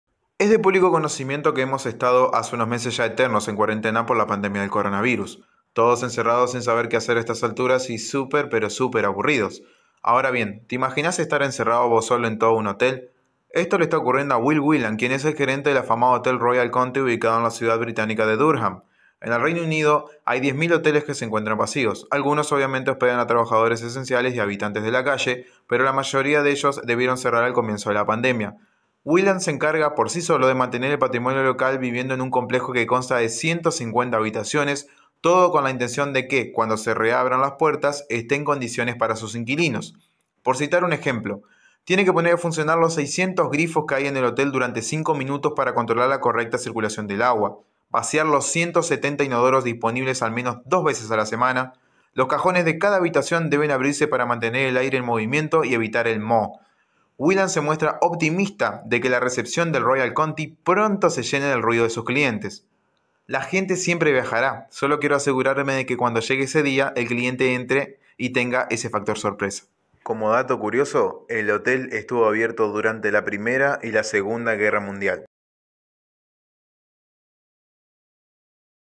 Dato Curioso: Vivir Solo En Un Hotel
Historia breve de la vida actual de un encargado de hotel en Durham,Reino Unido, durante la cuarentena.